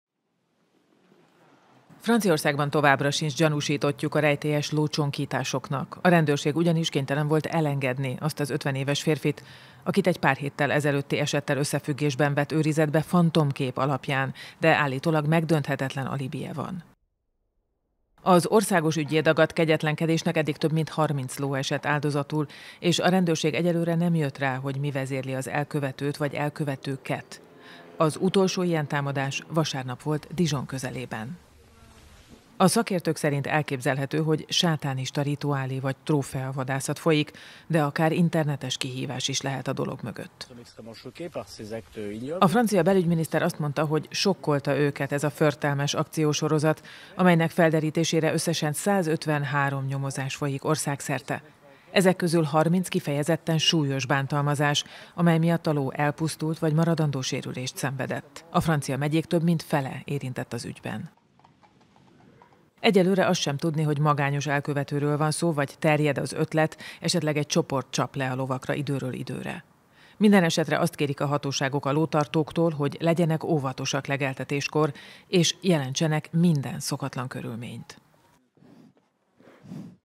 Elengedték a lócsonkítások gyanúsítottját Franciaországban
Az 50 éves férfinak sziklaszilárd alibije van. Úgyhogy továbbra is kérdés, ki csonkítja és öli a lovakat a francia megyék több mint felében. A bűnténysorozat pár hete kezdődött.